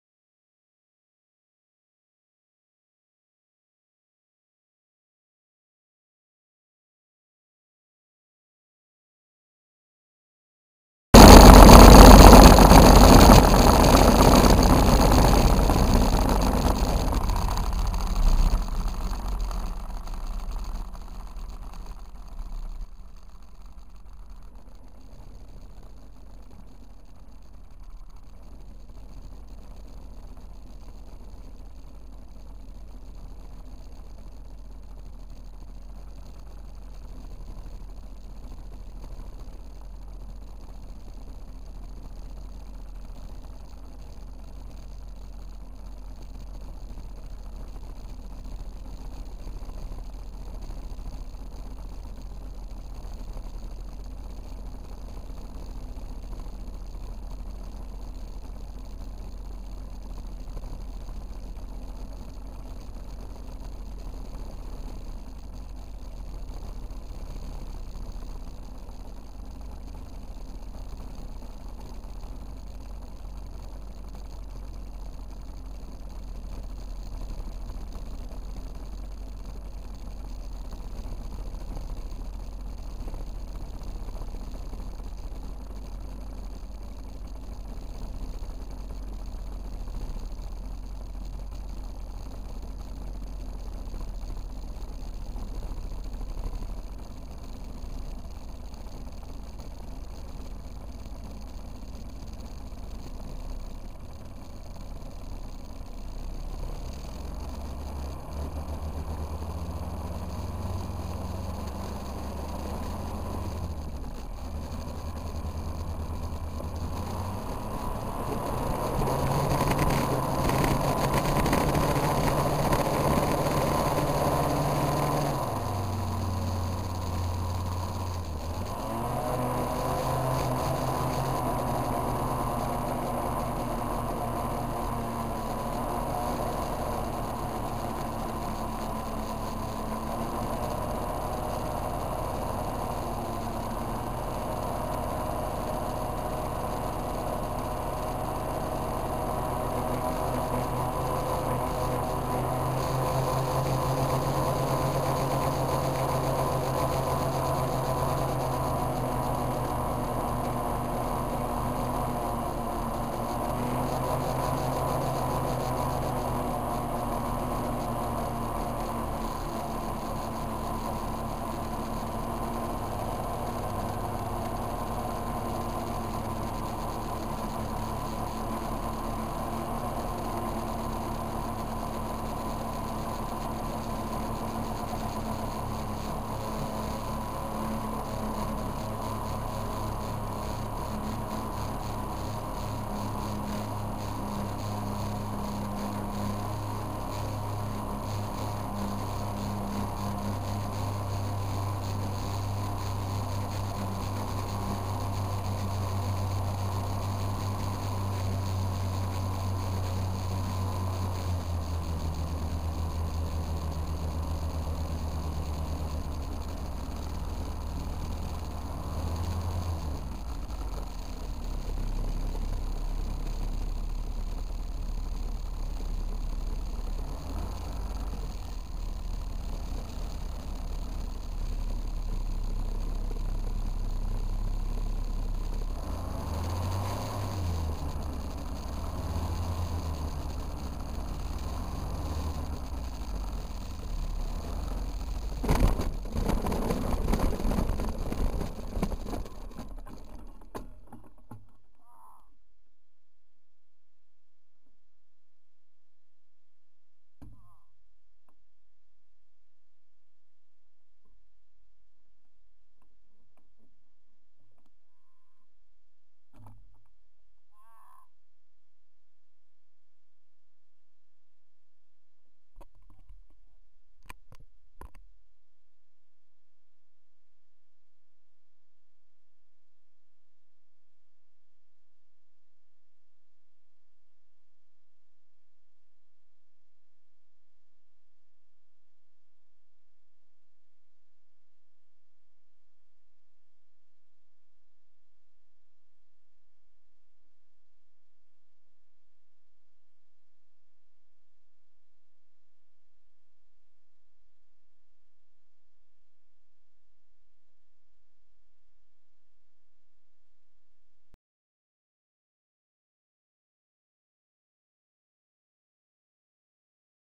first flight pm2 and a crash
this was my first time flying a powered parachute. i know I shouldn't have flown with the wind they way it was though, but i wanted to fly!